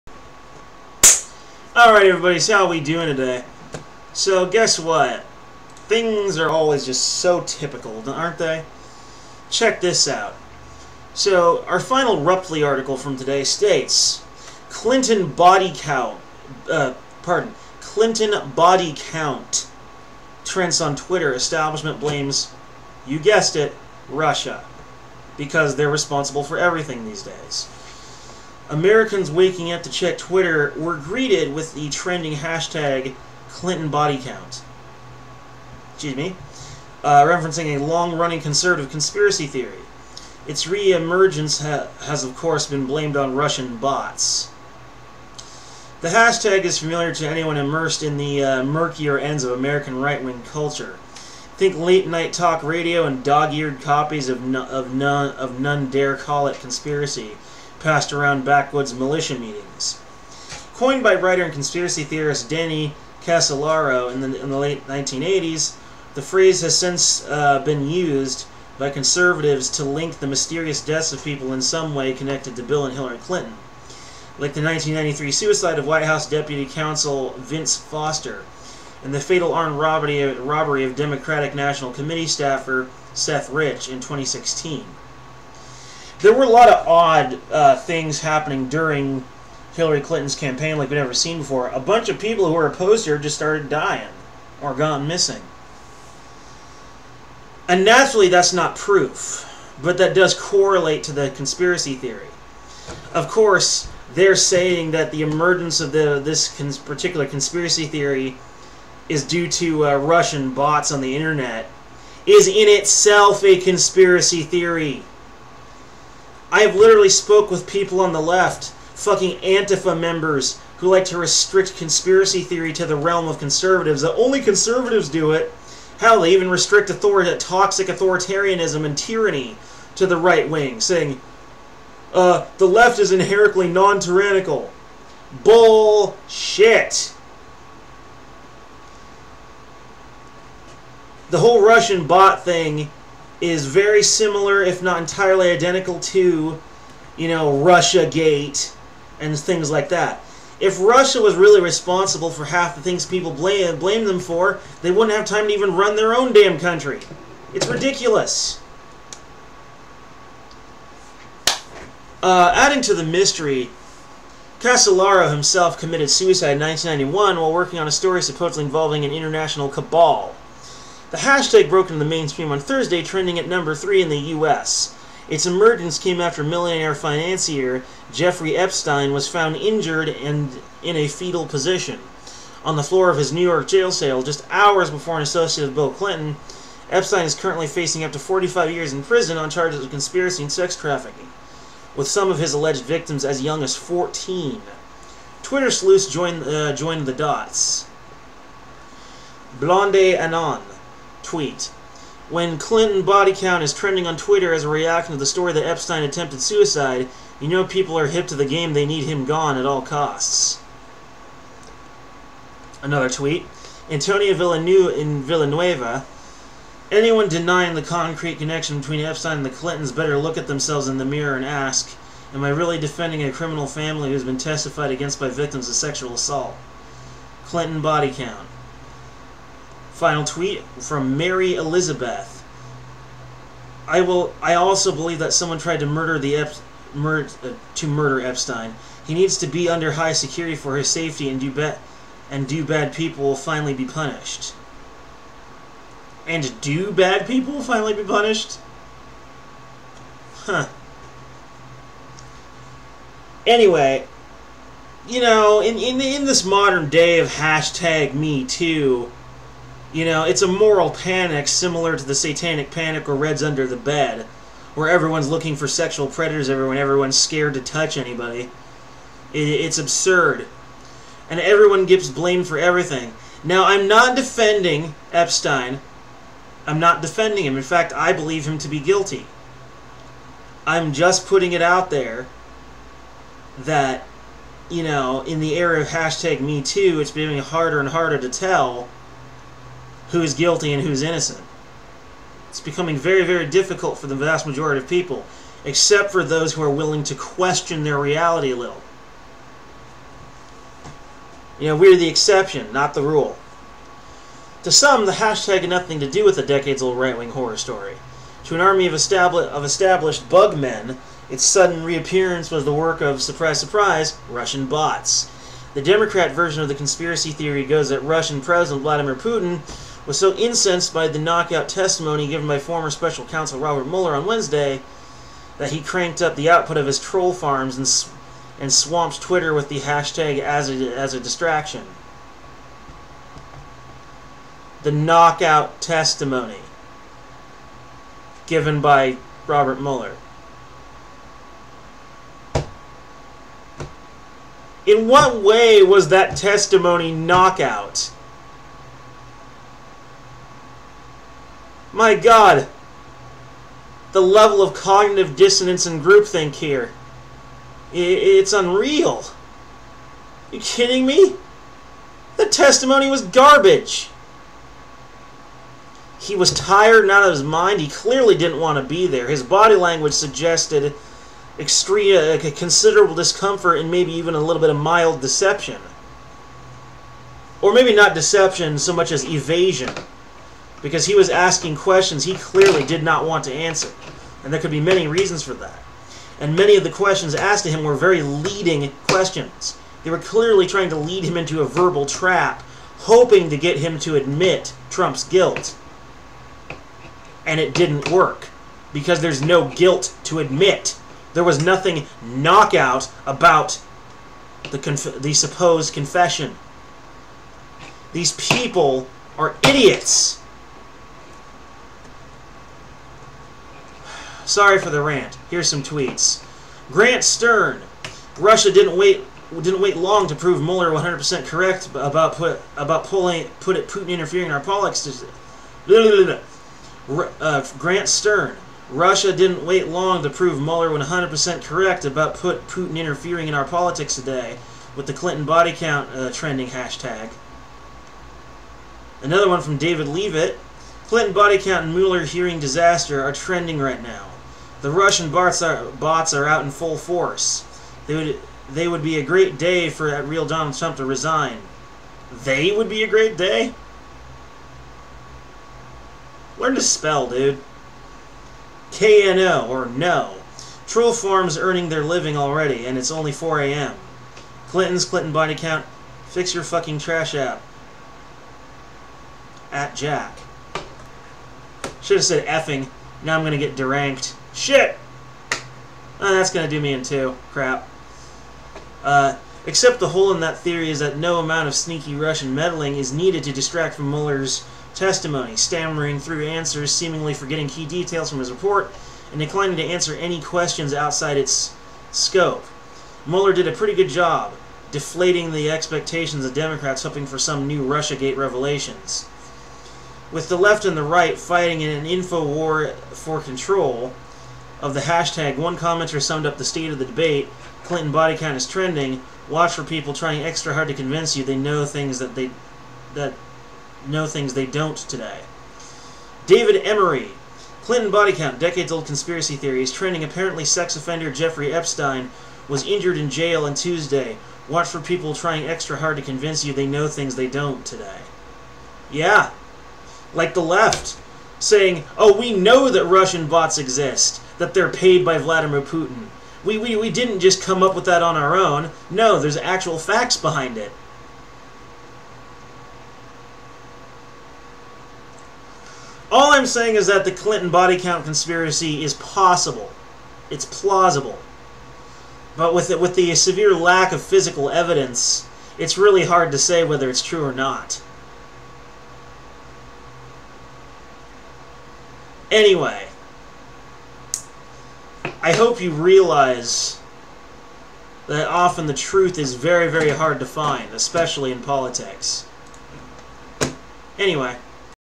Russian Bots blamed for clintonbodycount
Russian bots are a left wing conspiracy.